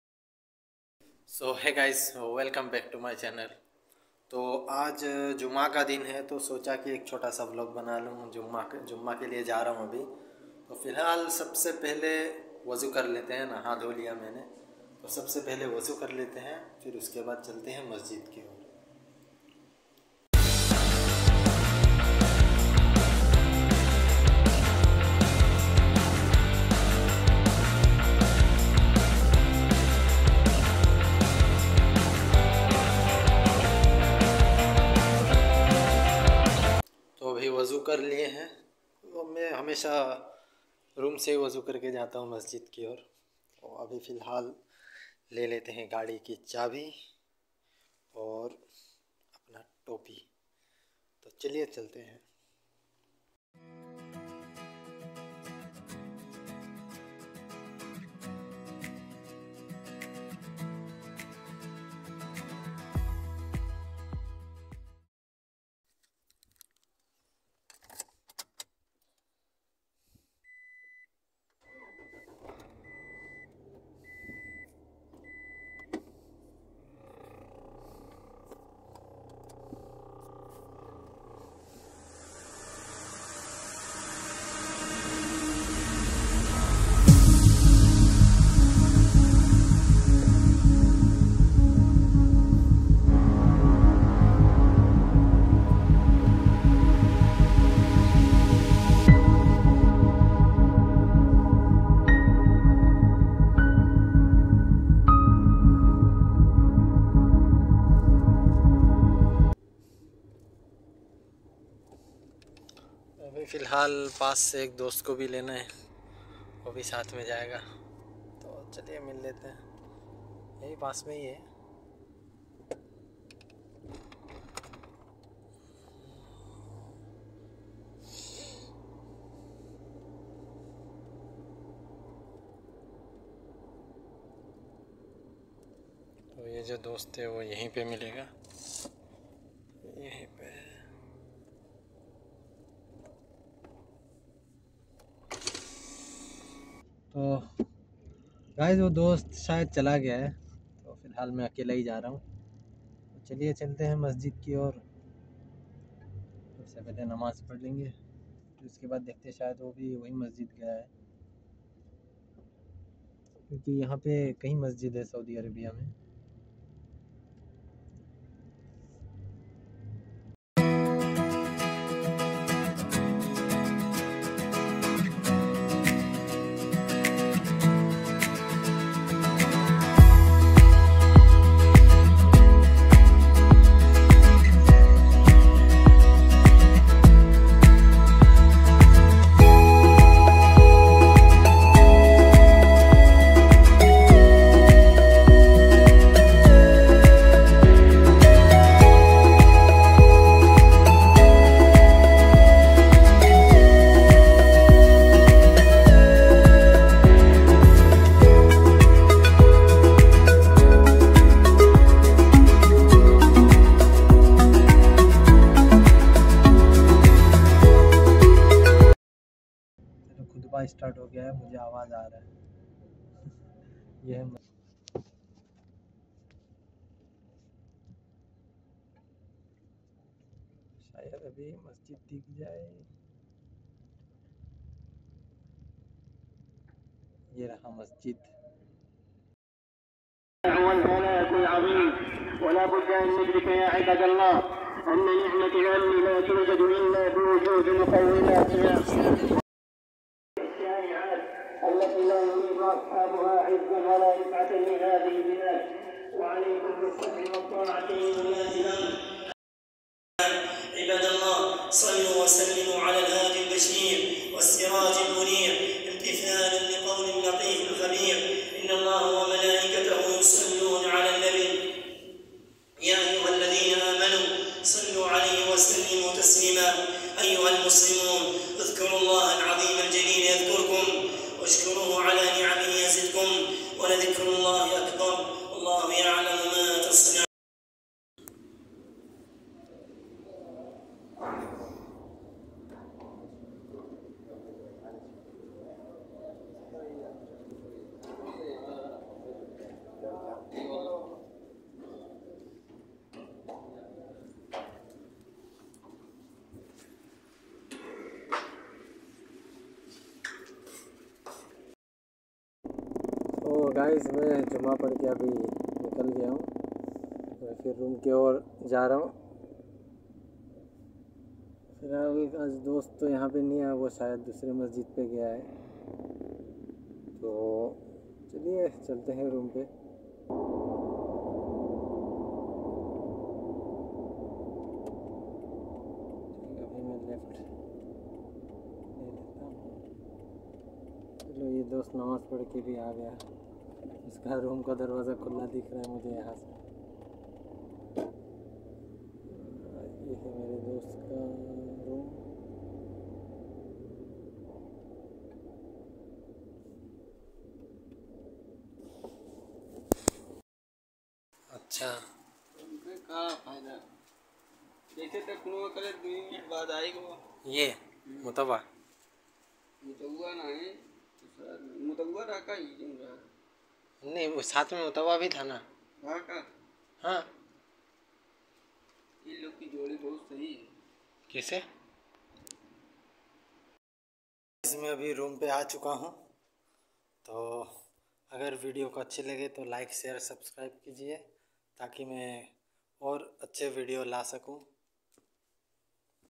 A Day in My Life: Jumaa Namaz Vlog from the Kingdom of Saudi Arabia | जुमा नमाज व्लॉग. Vlog 3
Daily vlog

In this video, we explore the significance and rituals of Jumaa Namaz in Saudi Arabia. Join us as we delve into the cultural and religious importance of the Friday prayer, highlighting the unique practices observed in various mosques across the kingdom. Witness the vibrant community spirit and devotion that characterizes this sacred gathering.